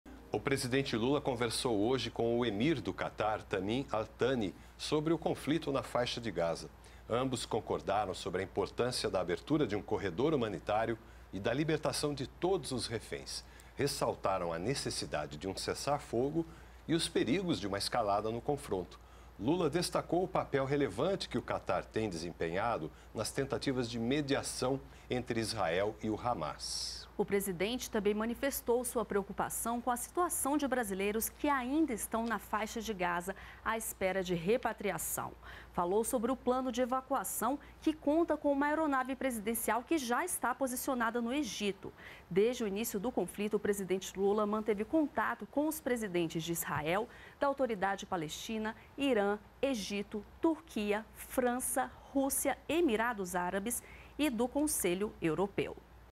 LULA CONVERSA COM EMIR DO CATAR SOBRE CONFLITO NA FAIXA DE GAZA
O presidente Lula conversou com o emir do Catar, Tamim Al-Thani, sobre o conflito na Faixa de Gaza. Ambos concordaram sobre a importância da abertura de corredor humanitário e da libertação dos reféns.